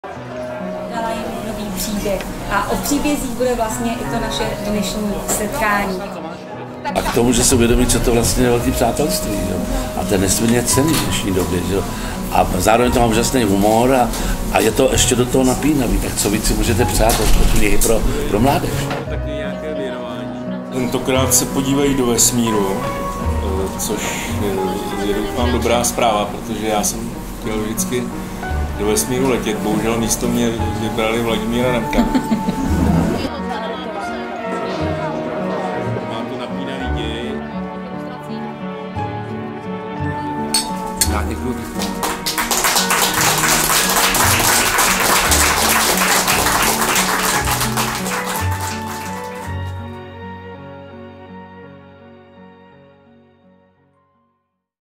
Křest knihy H2O a záhada Zlaté slzy
Knihu vypustili do světa herec Jiří Lábus spolu s autory – ilustrátorkou Galinou Miklínovou a spisovatelem Petrem Stančíkem. Křtilo se jak jinak než naší kohoutkovou. Skupina Veolia byla partnerem knihy.

Předchozí díly získaly ocenění Zlatá stuha 2018 v kategorii beletrie pro děti či Audiokniha roku 2020 a vyšly také ve Španělsku, ve Slovinsku nebo v Chorvatsku.